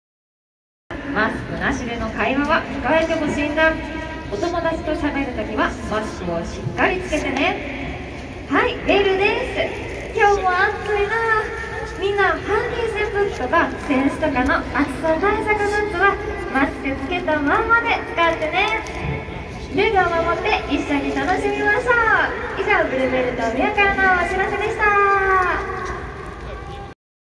またまたブルベルが喋るマスコットに(田畑実和さんver)
去年も喋りましたねw
山根七星さんverで

リポーターが代わっても喋るみたいです。笑

ブルベル随分声変わったなwww